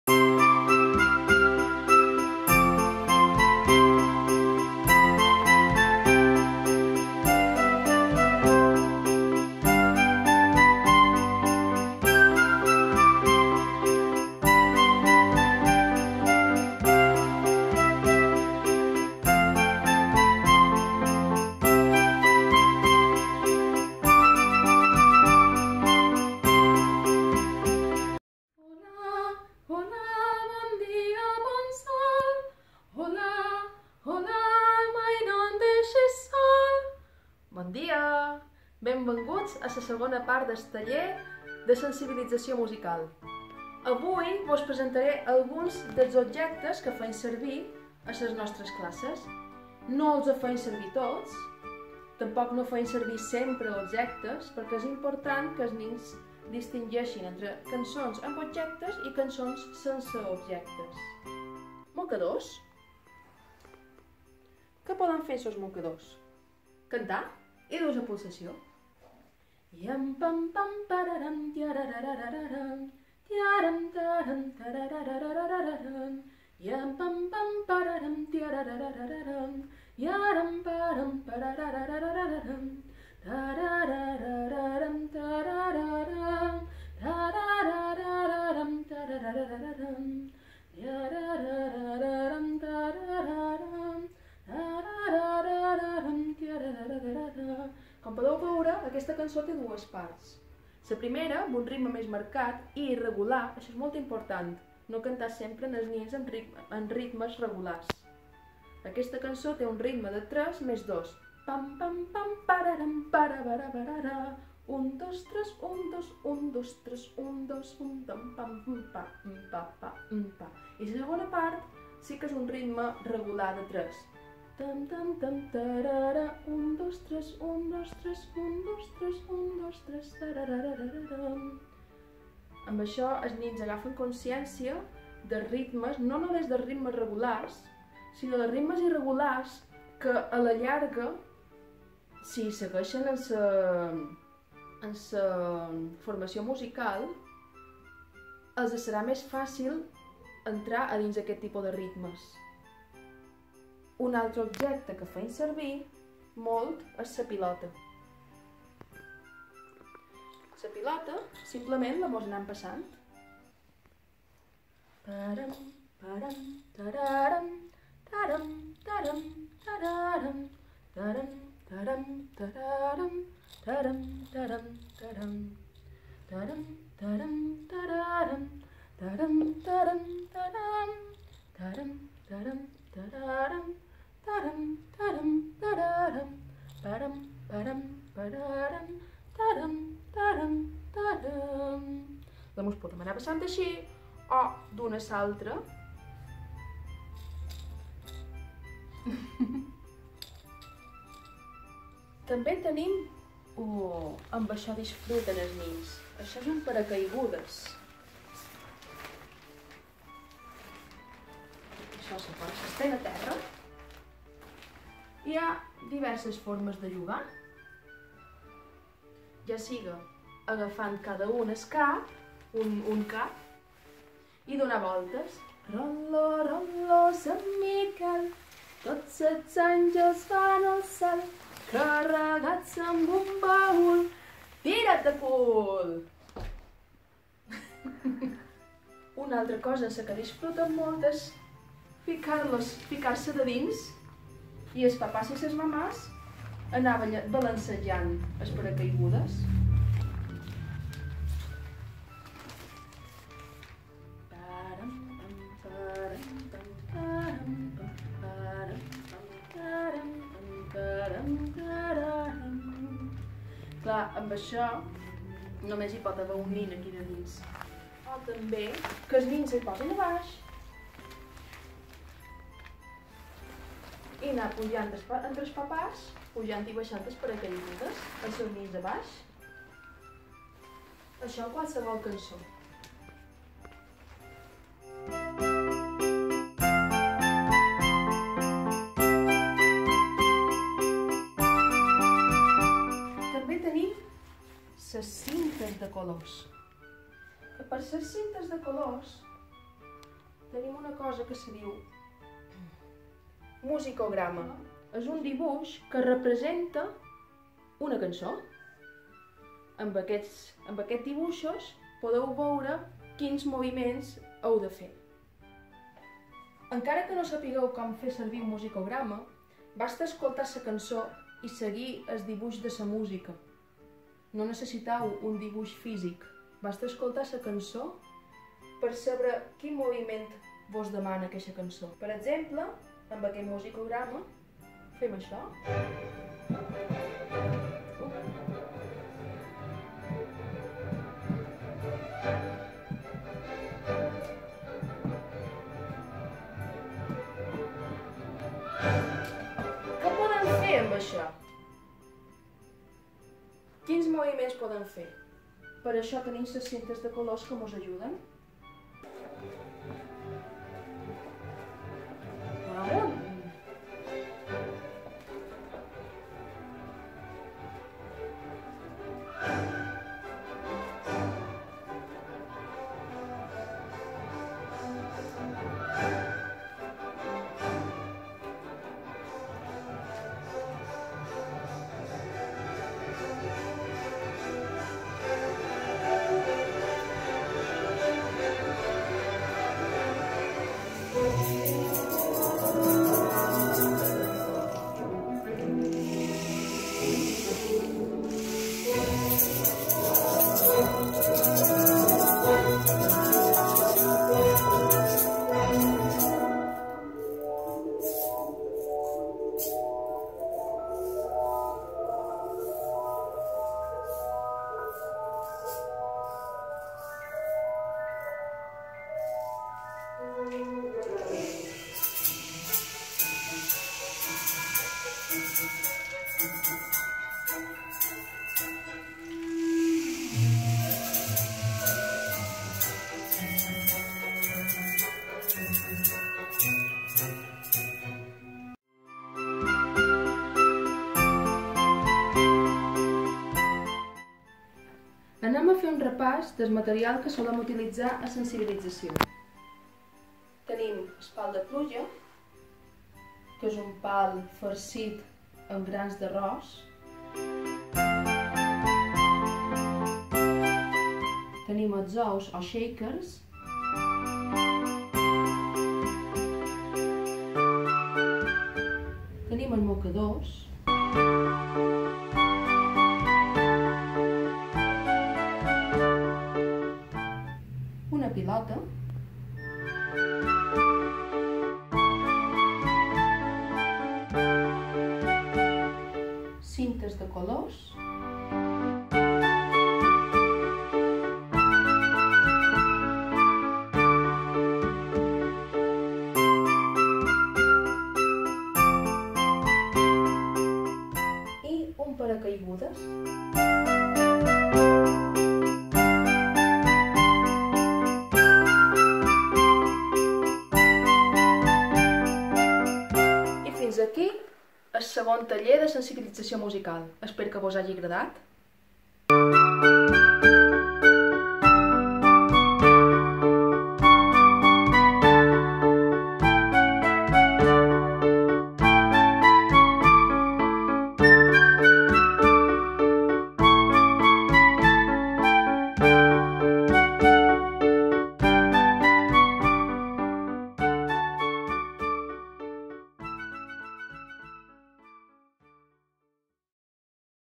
TALLER DE SENSIBILITZACIÓ MUSICAL - PART 2
Consisteix en la introducció de la música als més petits. D’aquesta manera es desenvolupa la capacitat auditiva, física, intel·lectual i emocional del nin, mitjançant jocs i manualitats.  A partir dels 2 anys. Dies 7,9 i 11 desembre.